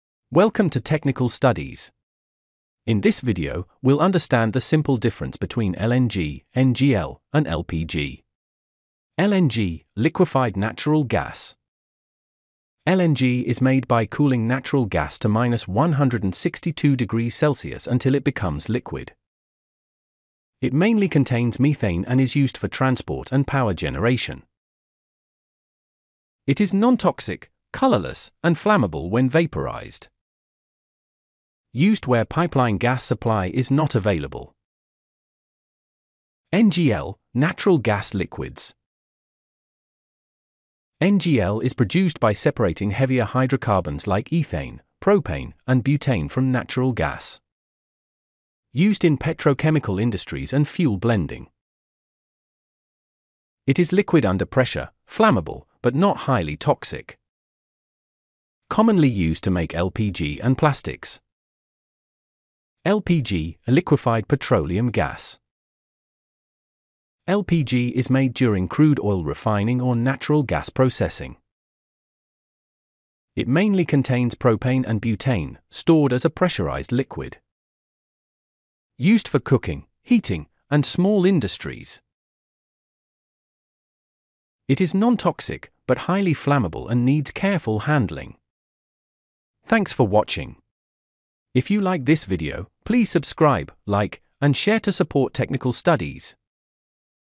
LNG, NGL & LPG Explained | Composition, Production and Uses
In this video, we explain the clear difference between LNG, NGL, and LPG in simple terms.
You’ll learn how each gas is made, what it contains, and where it is used.
We also discuss their production process, purpose, and safety aspects.
Perfect for engineers, students, and anyone in the oil and gas industry.